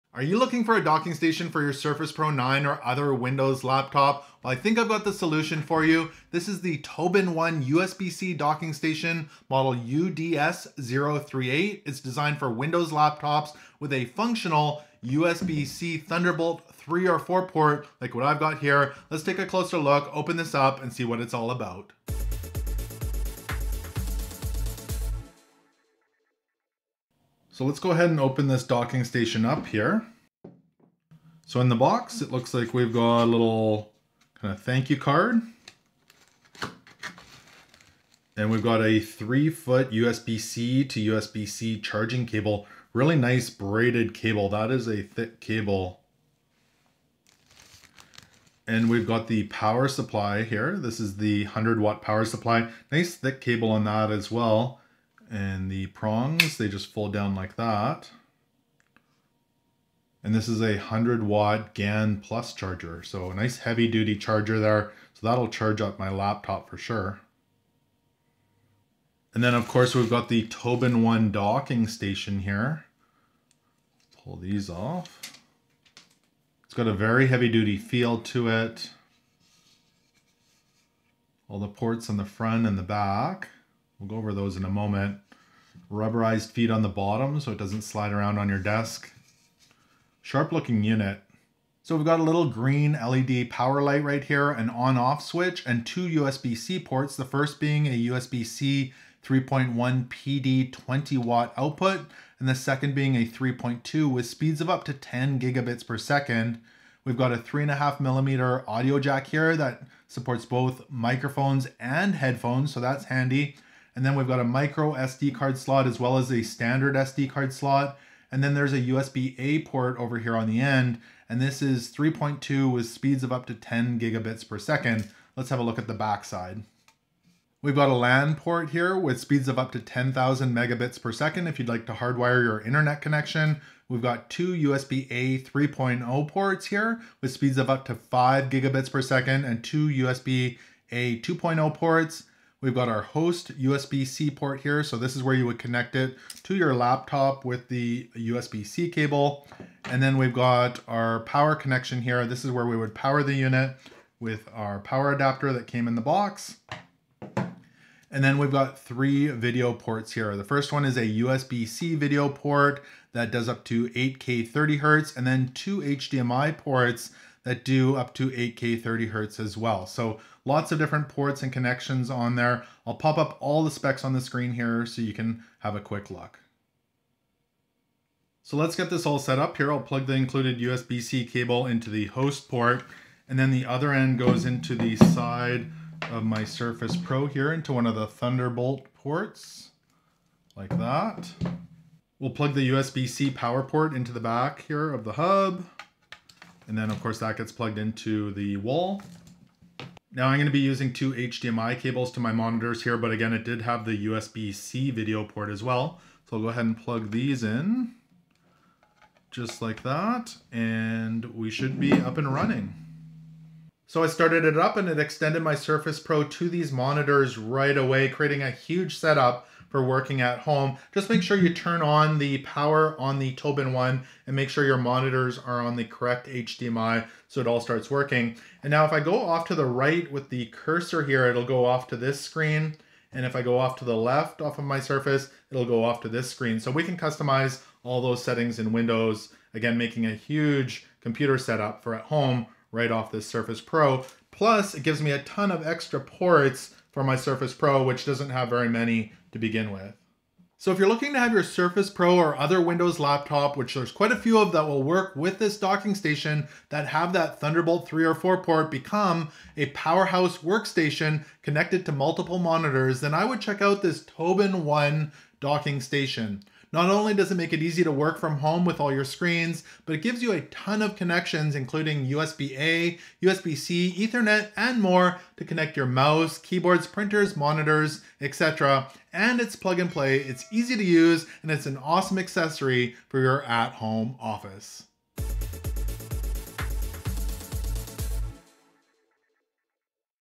Surface Pro Dock: TOBENONE Thunderbolt Docking Station - A Closer Look!
Level up your Surface Pro 9 with a single cable with this Surface Pro Dock!  The TobenONE Thunderbolt Docking Station is the ultimate companion for your Surface Pro (or many Thunderbolt 3/4 Windows laptops).  Unleash a desktop experience with 16 ports, including dual HDMI for gorgeous visuals, a USB-C display port for a triple monitor setup, blazing-fast data transfer speeds, and an SD card reader. Plus, a powerful 100W charger keeps your laptop powered and lets you charge your phone too! Edit videos, game like a pro, or dominate spreadsheets – the TobenONE Docking Station is your one-stop shop for a transformed workspace. Let's take a closer look and see this Thunderbolt docking station in action!

🛒TobenONE Docking Station on Amazon